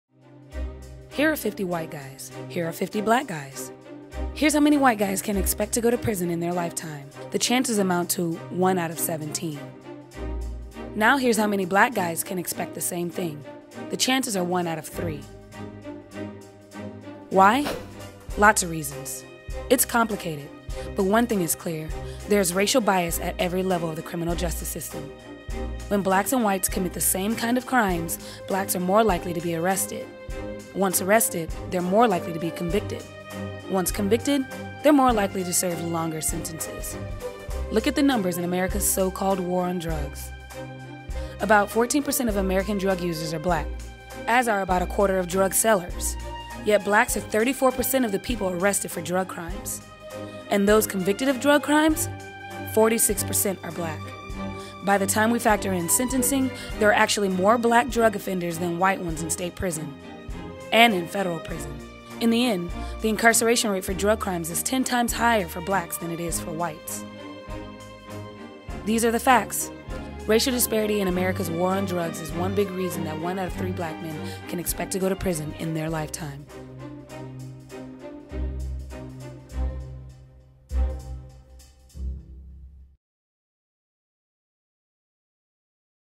What are the odds you'll go to prison? • BRAVE NEW FILMS: JUSTICE #1
What are the odds that you will go to prison?  1 out of 17 white guys wind up behind bars at some point.  How many black guys can expect the same thing?  1 out of 3.

More Black Men Are In Prison Today Than Were Enslaved In 1850

Why are there so many black people in prison?  Why the disparity?  Lots of reasons.  It's complicated.  But one thing is clear -there is racial bias at every level of the criminal justice system.  When blacks and whites commit the same kind of crimes, Blacks are more likely to be arrested.

Once arrested, they're more likely to be convicted.
Once convicted, they're more likely to serve longer sentences.

Look at the numbers in America's so-called "war on drugs".  About 14% of American drug user are black.  As are about a quarter of drug sellers.  Yet blacks are 34% of the people arrested for drug crimes.  Of those convicted of drug crimes, 46% are black.

By the time we factor in sentencing, there are actually more Black drug offenders than White ones in state prison, AND in Federal prison.  In the end, the incarceration rate for drug crimes is ten times higher for blacks than they are for whites.

These are the facts.

Racial disparity in America's "War on Drugs" is one big reason why 1 out of every 3 black men can expect to go to prison in their lifetime.

Want to learn more about how to fight our racist criminal justice system?  Sign up for our mailing list at